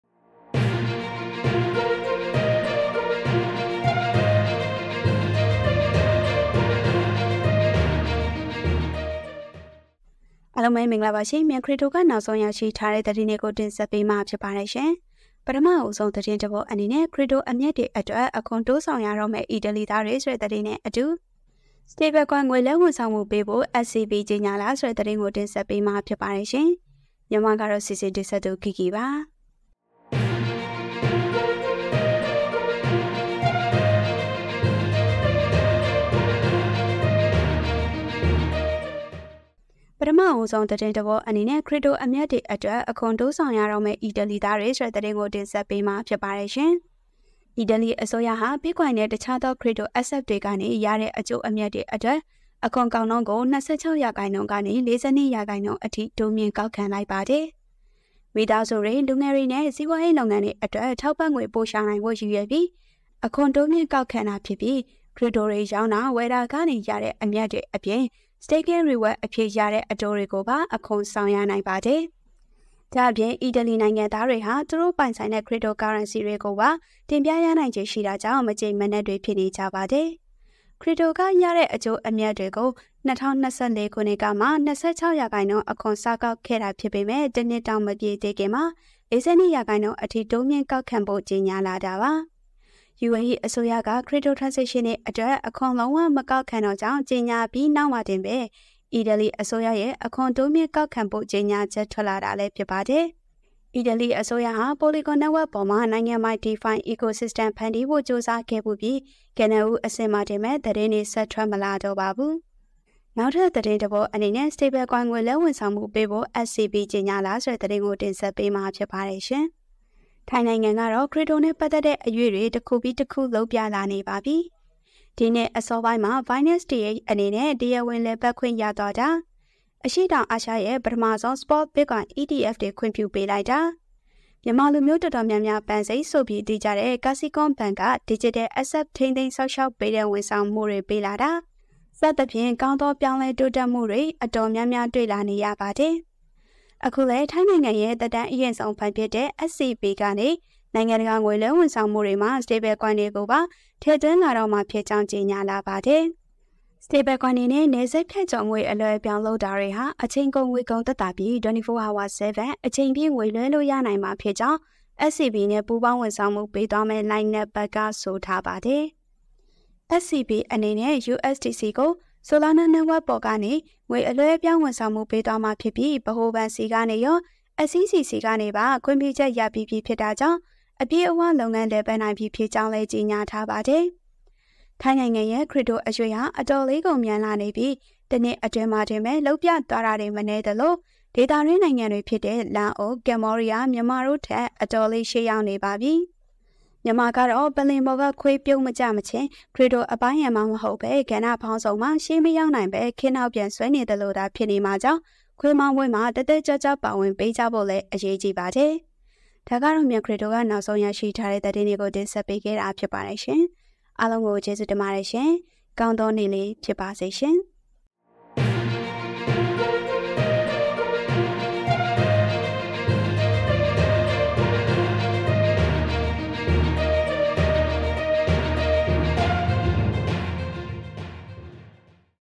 ထိုင်းနိုင်ငံရဲ့ နောက်ထပ် Crypto အရွေ့တခု | MCM Daily News (18th October 2024)
Crypto ကိုဘယ်ကစရမလဲ လေ့လာနေသူများအတွက် Study Room on Discord
Link to Myan Crypto Facebook